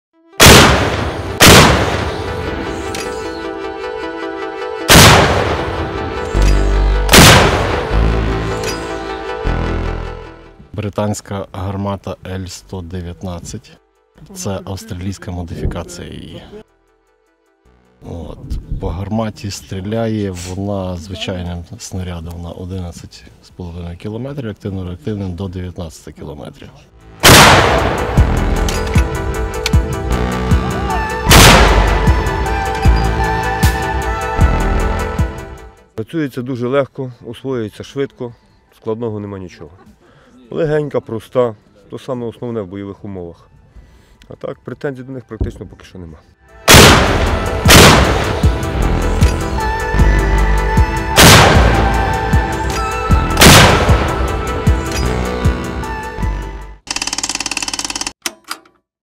Ukrainian Troops Target Russian Positions With British L119 Gun in Donbas Region
A Ukrainian artillery unit fires on Russian positions with a British L119 gun on the frontline in the Lugansk region. "If Western partners supply their artillery weapons and shells, this is immediately felt on the battlefield. We can adequately respond to the enemy and create conditions for advancing," says Oleksandr, commander of the unit.

n18oc_world      n18oc_crux